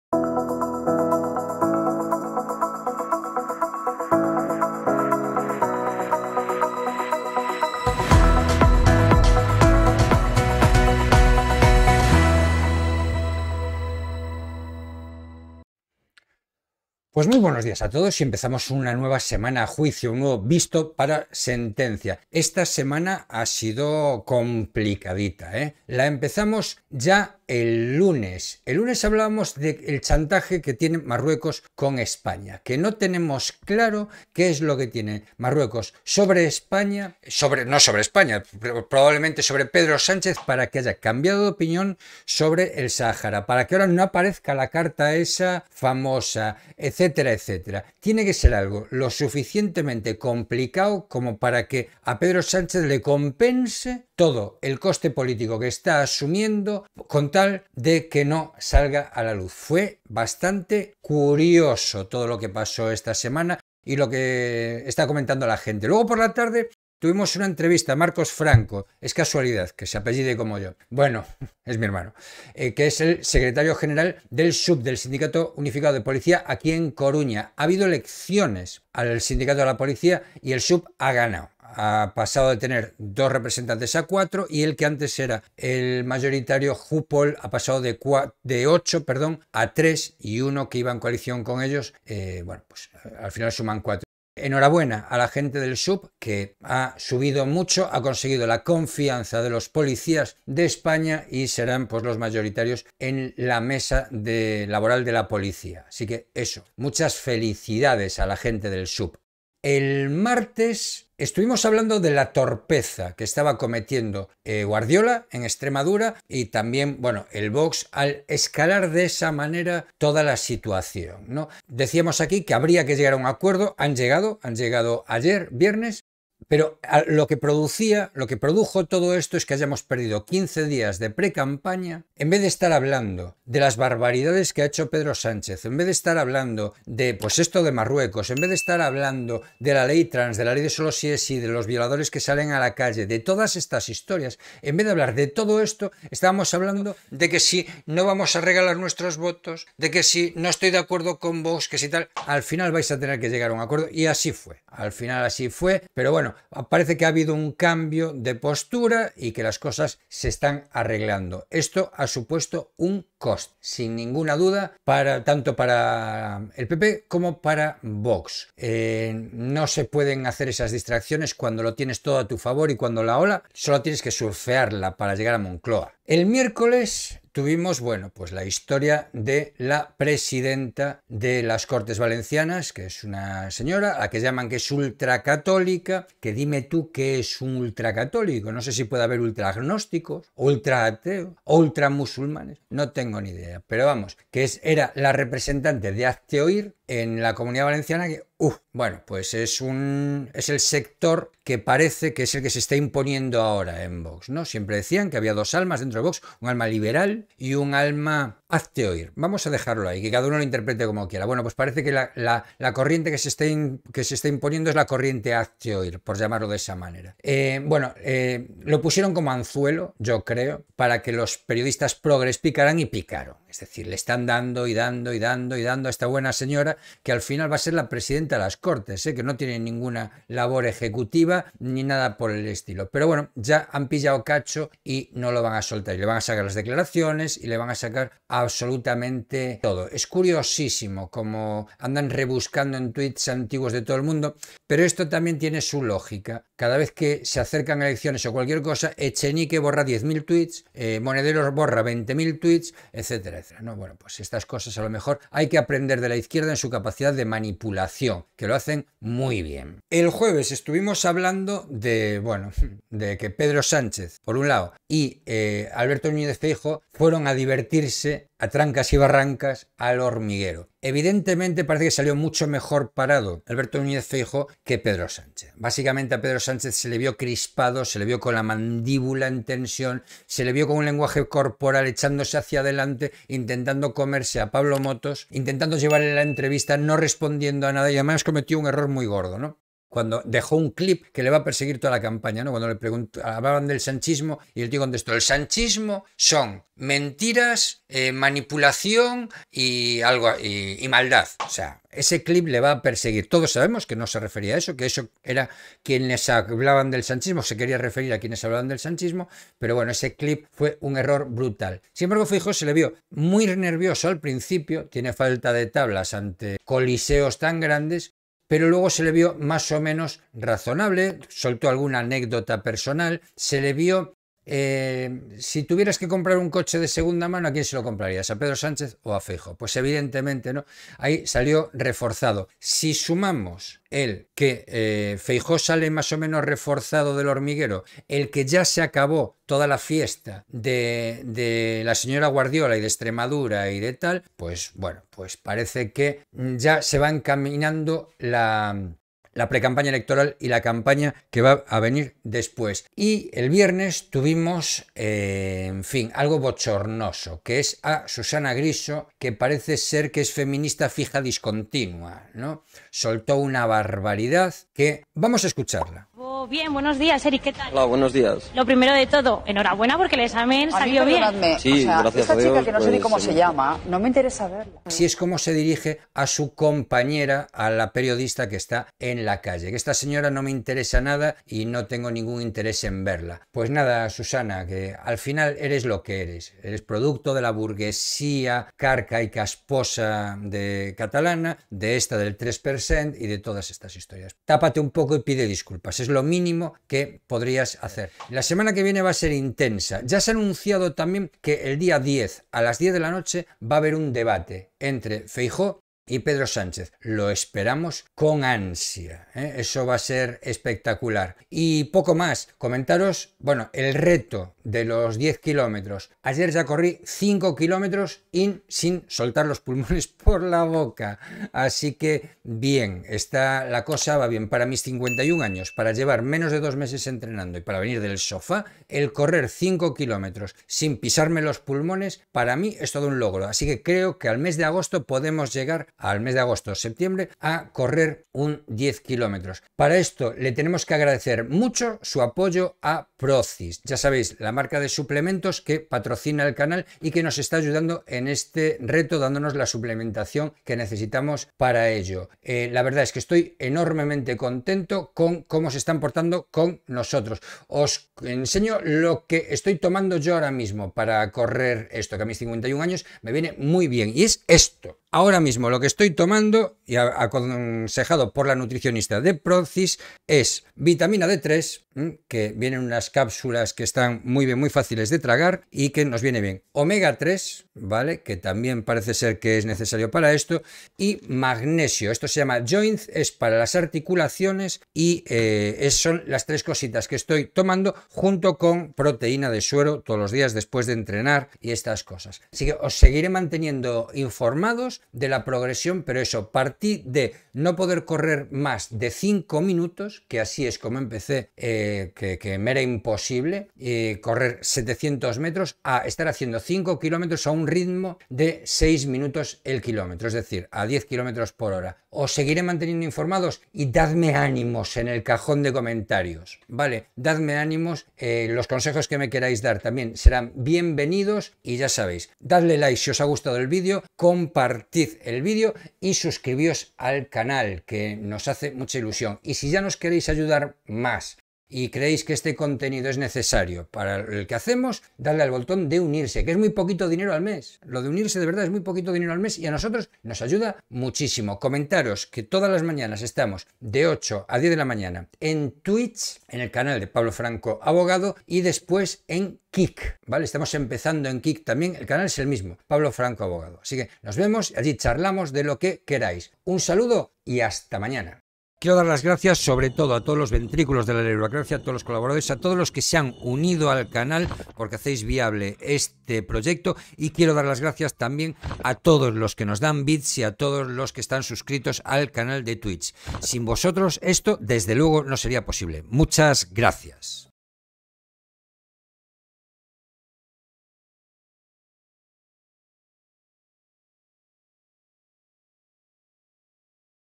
👨🏻‍⚖️LA SEMANA A JUICIO XI👨🏻‍⚖️PEDRO SÁNCHEZ, PP Y VOX , HAZTEOIR, EL HORMIGUERO, SUSANNA GRISO.
Analizamos desde un punto de vista jurídico la actualidad política.

Si te has perdido el directo en el canal morado, aquí tienes un canal donde subiremos todos los directos.

Edición: Lenskizo
Miniatura: Champuff